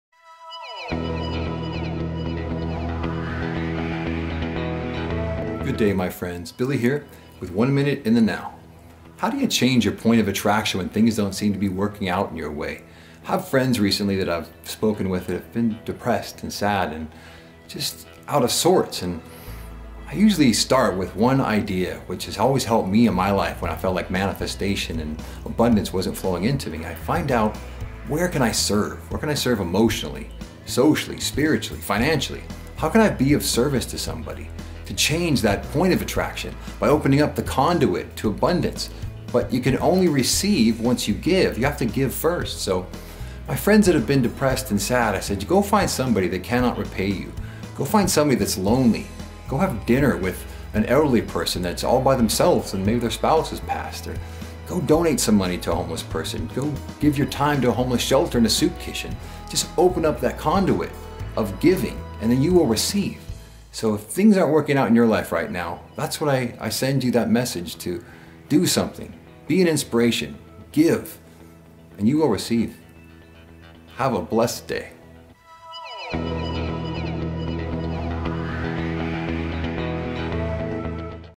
How to Change your Point of Attraction
In my life, when manifestation seems to be slowing down or at an all out stand still- Here is what I do...